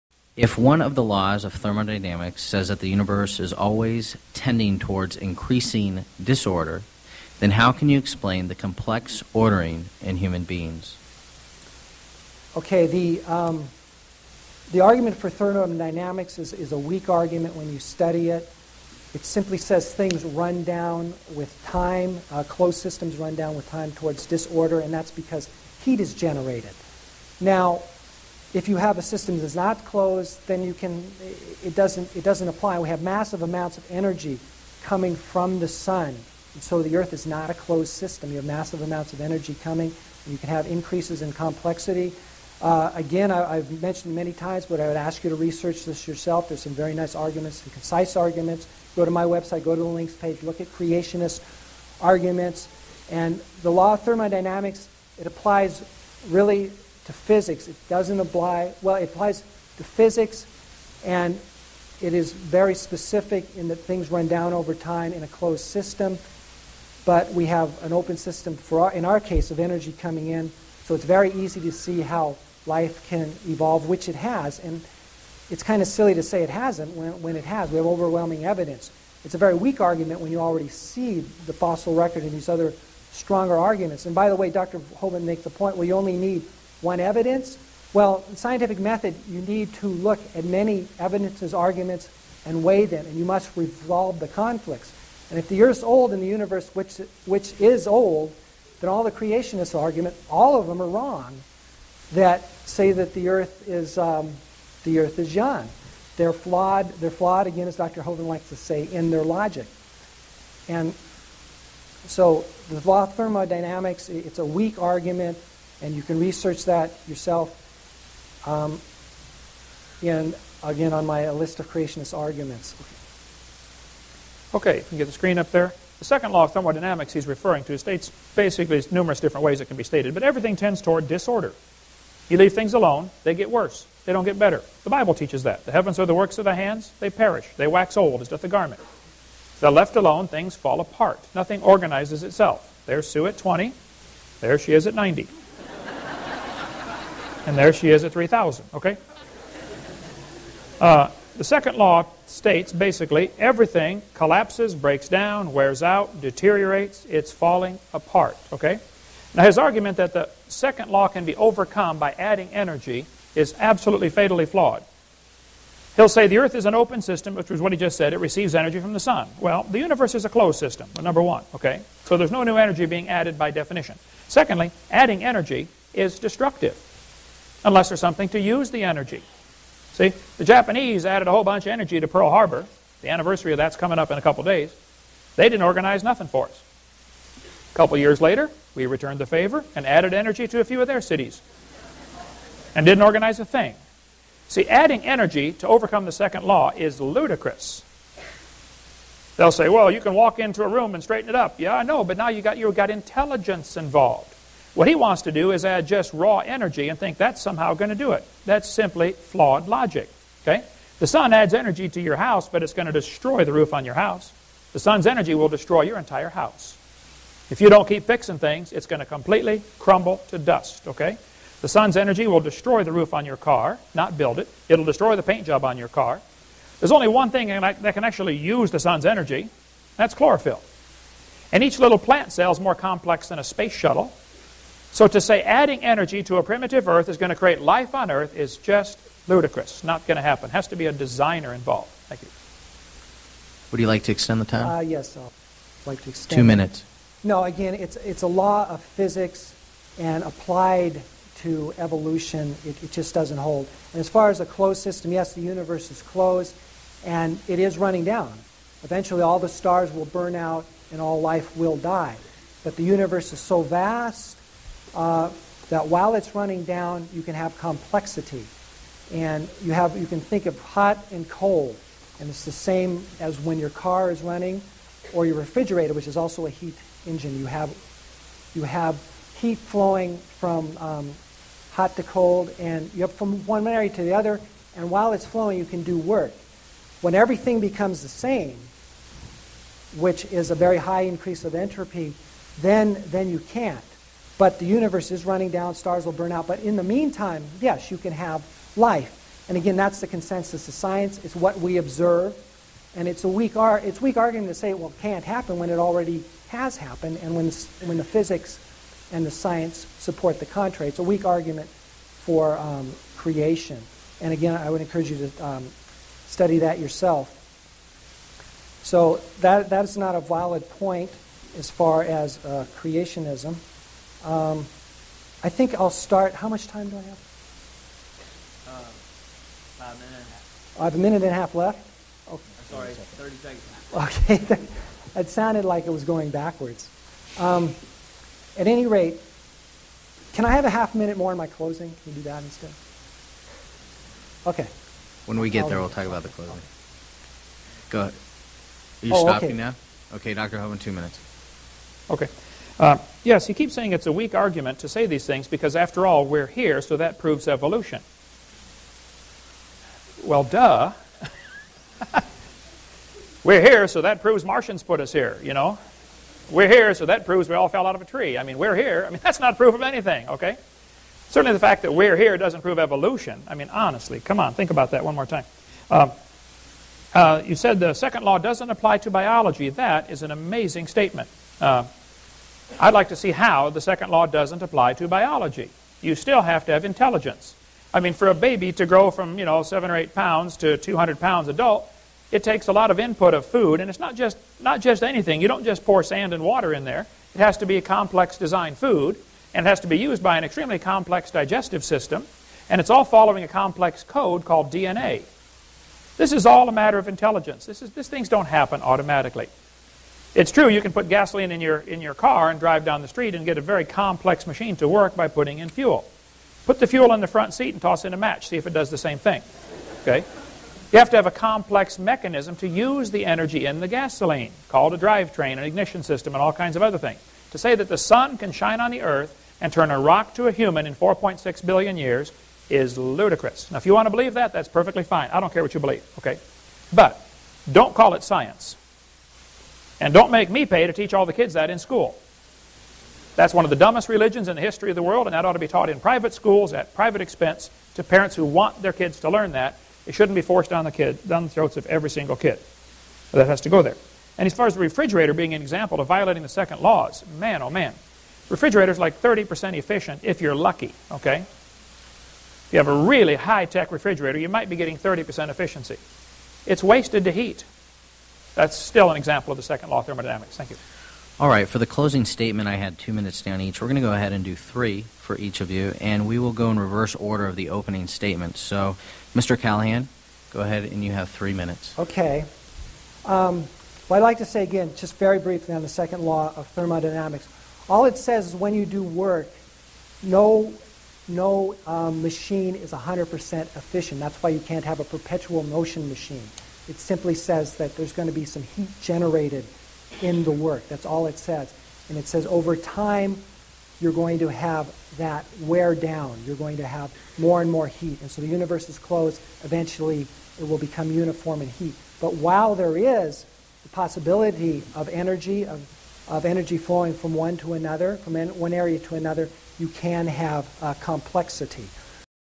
Hovind Debate v. Callahan (theistic evolution): Order
Part 13 of 14. The 2nd Law of Thermodynamics. Creation Science Versus Conventional Science. Evolution Observation. Young-Earth creationist Dr. Kent Hovind (drdino.com) debates theistic evolutionist John D. Callahan (faithreason.org), Dec-5-04.